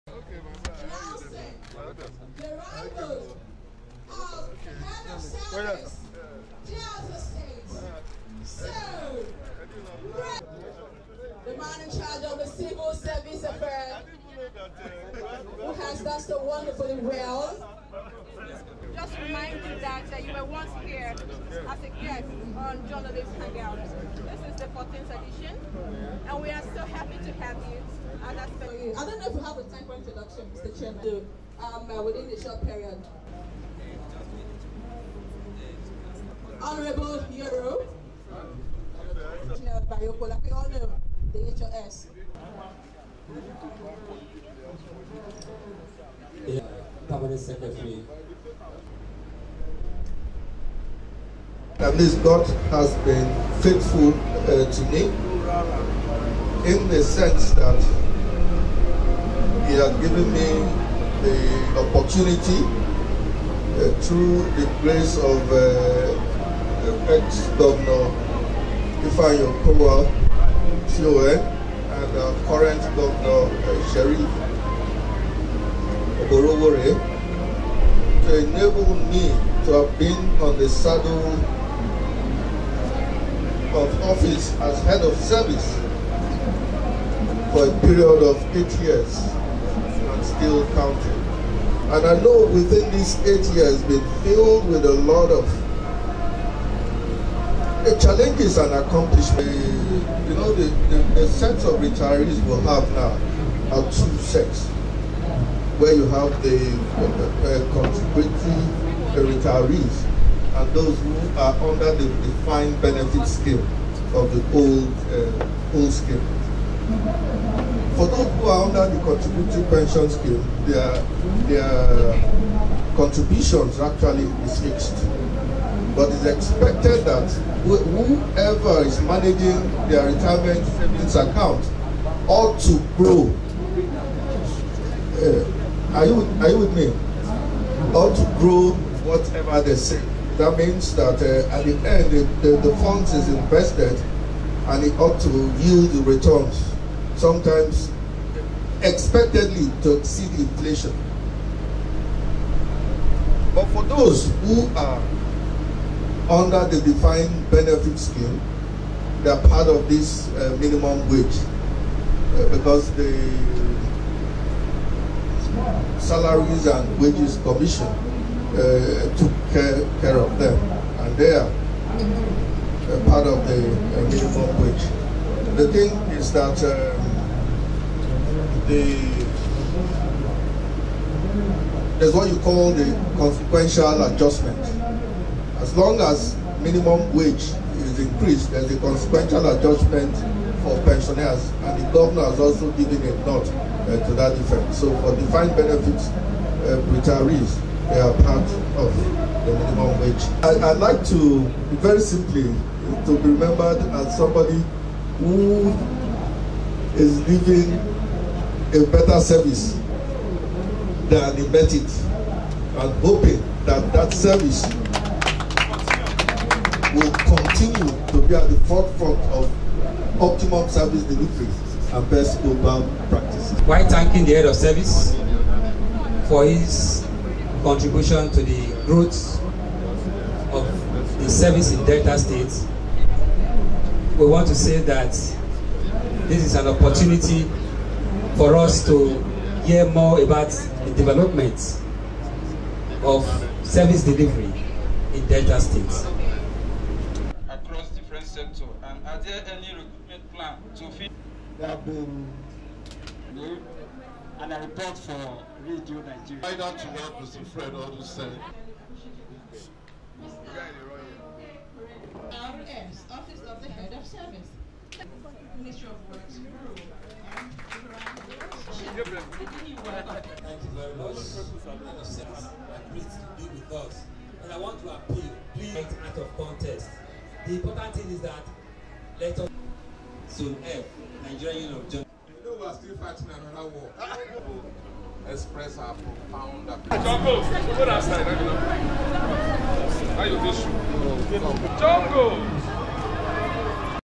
Minimum Wage; Delta Head Of Service Explains Implementation On Retirees In The State
As public servants across Nigeria benefit from the new minimum wage of 70,000 pasted into law by the Federal Government. Public servants in Delta State are enjoying a minimum wage of 77,500 however some retirees under the obnoxious Contributory Pension Scheme will not benefit from the new minimum wage package

The Delta State Head of Service, Reginald Bayoko, disclosed this while participating in a hangout with Journalists at the Press Centre of the Nigeria Union of Journalists (NUJ) in Asaba, the state capital.

According to Bayoko, contributions of retirees under the new scheme were fixed and could only be adjusted by the Pension Fund Managers trading with them.

He, however, explained that the case was different for retirees under the old Defined Benefit Scheme, pointing out that the National Salaries and Wages Commission would compute their consequential adjustment, which the State Governor, Sheriff Oborevwori, had approved for implementation.

On promotion arrears being paid with the new minimum wage, the HOS maintained that the issue of arrears had been put to rest in the state with the release of 5.5 Billion Naira by the Sheriff Oborevwori led state government to settle the backlog after which the MDAs were directed to ensure that officers were promoted as and when due

Mr. Bayoko, however, acknowledged the systemic challenges with the promotion of teachers, but assured that the minimum wage would not affect their promotion arrears

Quest Charles Kosipre reports that Earlier, the Chairman, NUJ Delta State, Comrade Churchill Oyowe had welcomed the State Head of Service to the Hangout and the development process of public service delivery in Delta State.